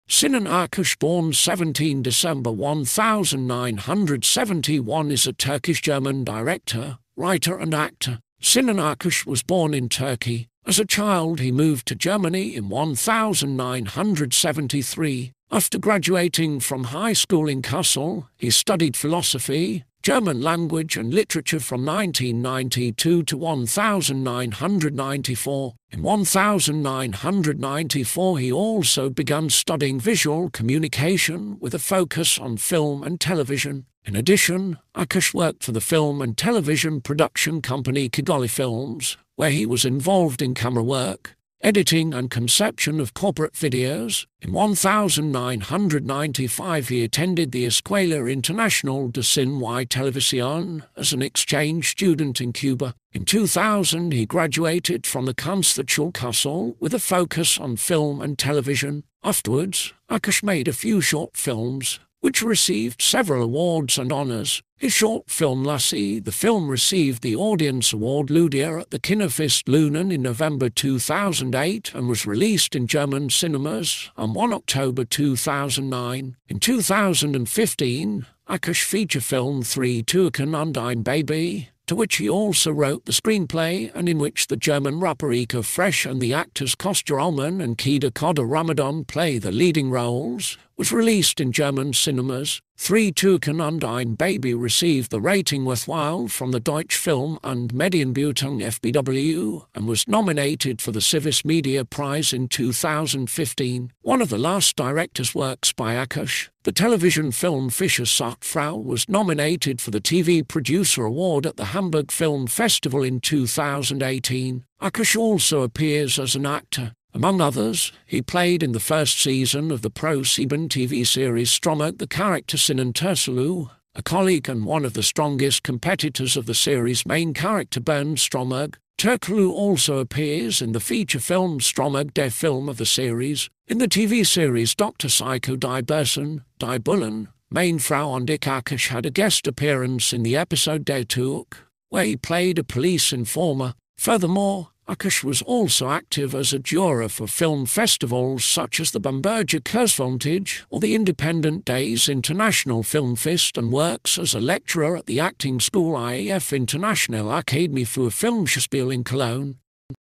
Sinan Akkuş Biography
This video is about Sinan Akkuş Biography in English. Sinan Akkuş (born 17 December 1971) is a Turkish-German director, writer and actor.
Sinan Akkuş was born in Turkey. As a child he moved to Germany in 1973. After graduating from high school in Kassel, he studied philosophy, German language and literature from 1992 to 1994. In 1994 he also began studying visual communication with a focus on film and television.
In addition, Akkuş worked for the film and television production company kigali-films, where he was involved in camera work, editing and conception of corporate videos. In 1995 he attended the Escuela Internacional de Cine y Televisión as an exchange student in Cuba. In 2000 he graduated from the Kunsthochschule Kassel with a focus on film and television.
Afterwards, Akkuş made a few short films, which received several awards and honours. His short film Lassie ( The film received the audience award Lüdia at the Kinofest Lünen in November 2008 and was released in German cinemas on 1 October 2009.
In 2015, Akkuş' feature film 3 Türken und ein Baby, to which he also wrote the screenplay and in which the German rapper Eko Fresh and the actors Kostja Ullmann and Kida Khodr Ramadan play the leading roles, was released in German cinemas. 3 Türken und ein Baby received the rating worthwhile from the Deutsche Film- und Medienbewertung (FBW) and was nominated for the Civis Media Prize in 2015. One of the last director's works by Akkuş, the television film Fischer sucht Frau was nominated for the TV Producer Award at the Hamburg Film Festival in 2018.
Akkuş also appears as an actor. Among others, he played in the first season of the ProSieben - TV series Stromberg the character Sinan Turçulu, a colleague and one of the strongest competitors of the series main character Bernd Stromberg. Turculu also appears in the feature film Stromberg - Der Film of the series. In the TV series Dr. Psycho – Die Bösen, die Bullen, meine Frau und ich Akkuş had a guest appearance in the episode Der Türke, where he played a police informer.
Furthermore, Akkuş was also active as a juror for film festivals such as the Bamberger Kurzfilmtage or the Independent Days International Filmfest and works as a lecturer at the acting school IAF - Internationale Akademie für Filmschauspiel in Cologne.